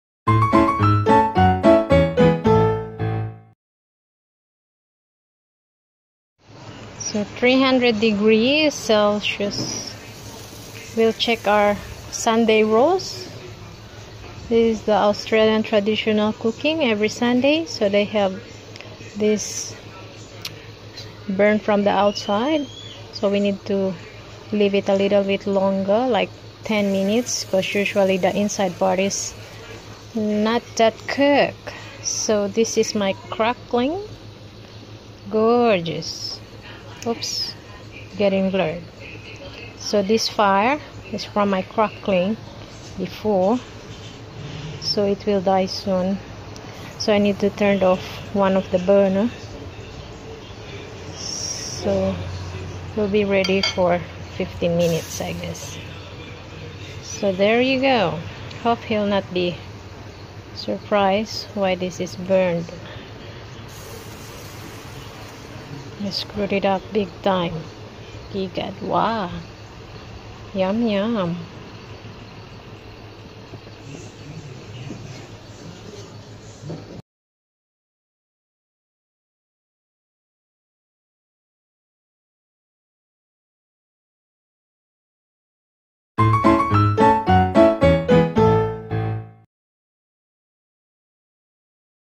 How to cook Beef Roast
Beef Roast pre marinated and slow-cooked for 2hrs with 300°c in a Barbecue oven.
Pork Crackling cook for 20 minutes.